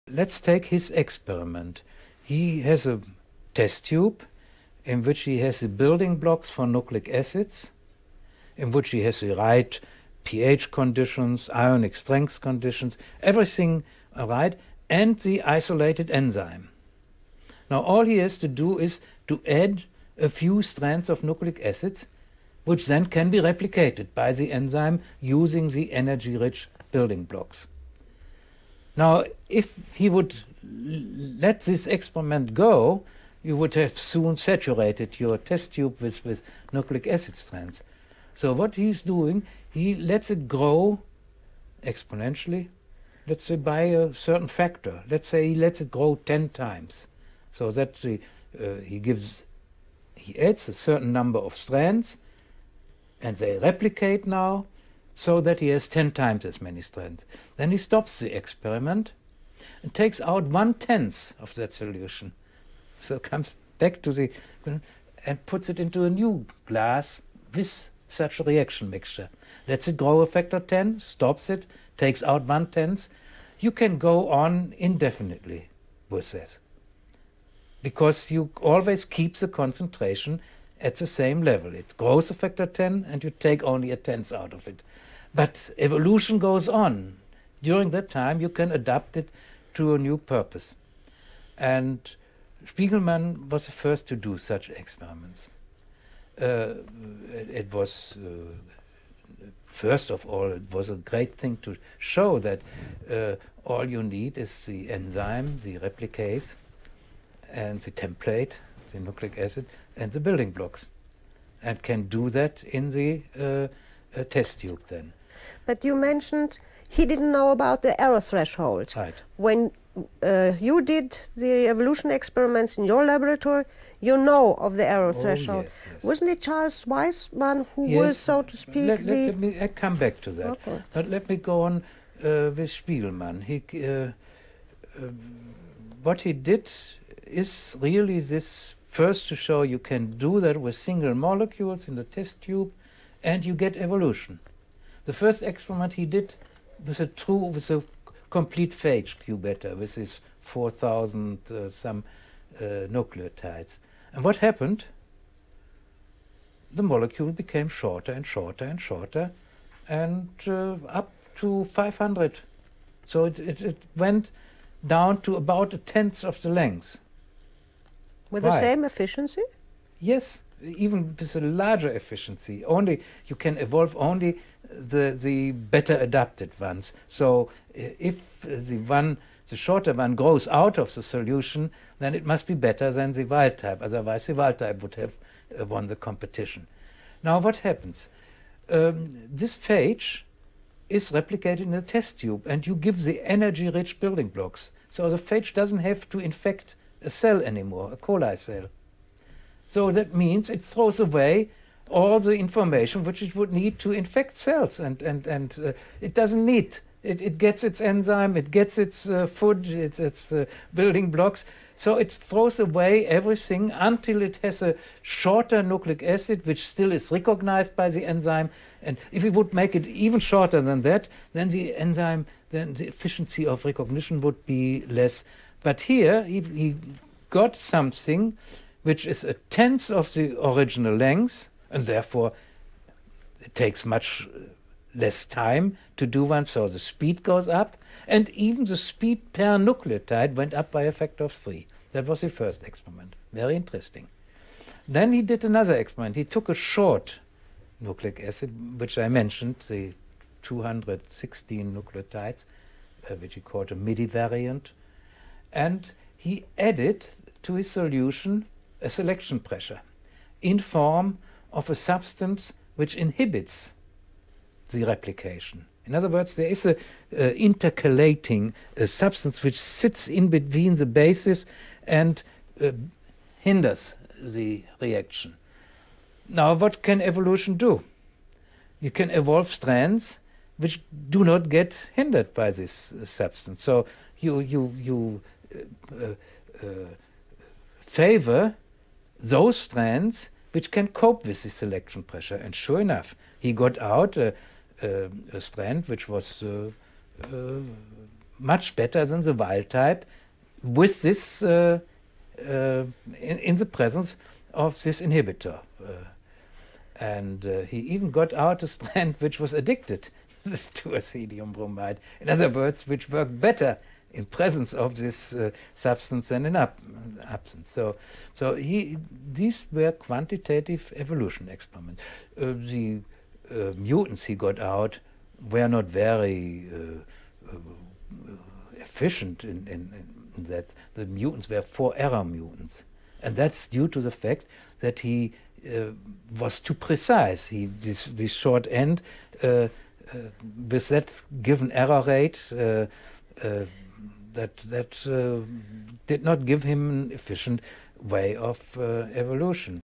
Manfred Eigen - Sol Spiegelman's quantitative evolution experiments (46/113)
Manfred Eigen (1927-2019) was a German biophysical chemist, best known for his work on measuring fast chemical reactions called relaxation techniques. He was co-recipient of the 1967 Nobel Prize in Chemistry. [Listener: Ruthild Winkler-Oswatitch; date recorded: 1997]

TRANSCRIPT: Let's take his experiment. He has a test tube in which he has the building blocks for nucleic acids, in which he has the right pH conditions, ionic strength conditions, everything are right, and the isolated enzyme. Now all he has to do is to add a few strengths of nucleic acids which then can be replicated by the enzyme using the energy-rich building blocks. Now if he would let this experiment go, you would have soon saturated your test-tube with nucleic acid strengths, so what he is doing, he lets it grow exponentially, let's say by a certain factor, let's say he lets it grow ten times, so that he gives... he adds a certain number of strengths and they replicate now so that he has ten times as many strengths. Then he stops the experiment and takes out one-tenth of that solution, so comes back to the... and puts it into a new glass with such a reaction mixture. Lets it grow a factor 10, stops it, takes out one-tenth. You can go on indefinitely with that. Because you always keep the concentration at the same level, it grows a factor 10 and you take only a tenth out of it. But evolution goes on. During that time you can adapt it to a new purpose, and Spiegelman was the first to do such experiments.

First of all it was a great thing to show that all you need is the enzyme, the replicase, and the template ‒ the nucleic acid ‒ and the building blocks, and can do that in the test tube then.

[RW-O] But you mentioned he didn't know about the error threshold.

That's right.

[RW-O] When you did the evolutionary experiments in your laboratory you know of the error threshold. Wasn't it Charles Weissmann who was, so to speak, the...?

Let me... I come back to that. But let me go on with Spiegelman. What he did is really the first to show you can do that with single molecules in the test tube and you get evolution. The first experiment he did was a complete phage Q-beta with his four thousand and some nucleotides, and what happened... the molecule became shorter and shorter and shorter, up to five hundred, so it went down to about a tenth of the length. Why?

[RW-O] With the same efficiency?

Yes, even with a larger efficiency. Only... you can evolve only the better adapted ones. So if the one... the shorter one grows out of the solution then it must be better than the wild type, otherwise the wild type would have won the competition. Now what happens? This phage is replicated in a test tube and you give the energy-rich building blocks, so the phage doesn't have to infect a cell any more, a coli cell.  So that means it throws away all the information which it would need to infect cells, and it doesn't need... it gets its enzyme, it gets its food, its building blocks, so it throws away everything until it has a shorter nucleic acid which still is recognised by the enzyme and...  If you would make it even shorter than that, then the enzyme... then the efficiency of recognition would be less. But here he got something which is a tenth of the original length and therefore takes much less time to do one, so the speed goes up, and even the speed per nucleotide went up by a factor of three. That was the first experiment... very interesting.

Then he did another experiment. He took a short nucleic acid, which I mentioned... the 216 nucleotides which he called a midi variant... and he added to his solution a selection pressure, in form of a substance which inhibits the replication. In other words there is an interchelating substance which sits in between the bases and hinders the reaction. Now what can evolution do? You can evolve strands which do not get hindered by this substance, so you favour those strands which can cope with the selection pressure, and sure enough he got out a strand which was much better than the wild type, with this... in the presence of this inhibitor. And he even got out a strand which was addicted to ethidium bromide, in other words which worked better in presence of this substance than in absence.

These were quantitative evolution experiments. The mutants he got out were not very efficient, in that the mutants were for error mutants, and that's due to the fact that he was too precise... the short end with that given error rate did not give him efficient way of evolution.